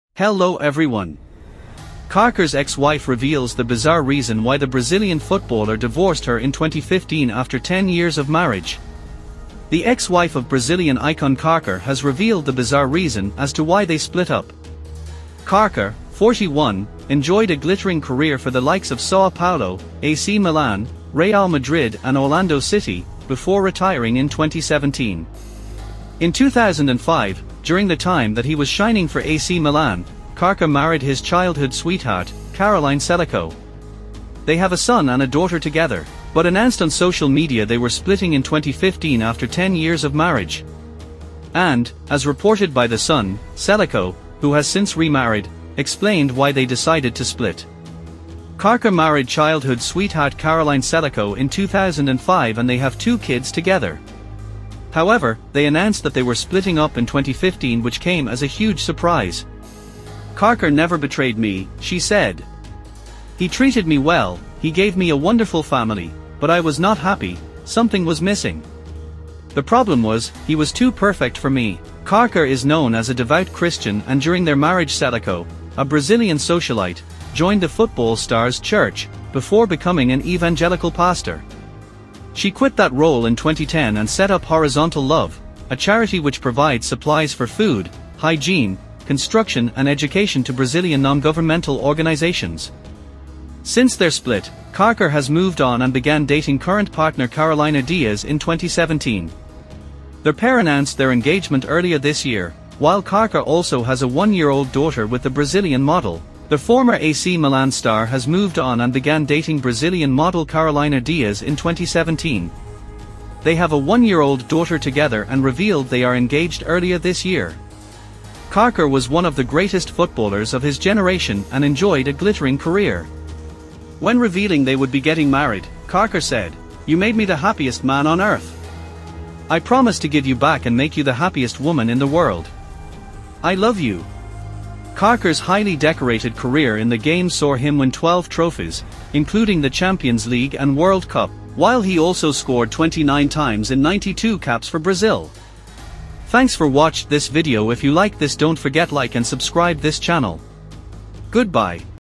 Kaka's ex-wife reveals the bizarre reason why the Brazilian footballer divorced
👋 Hello guys, welcome to a new YouTube video.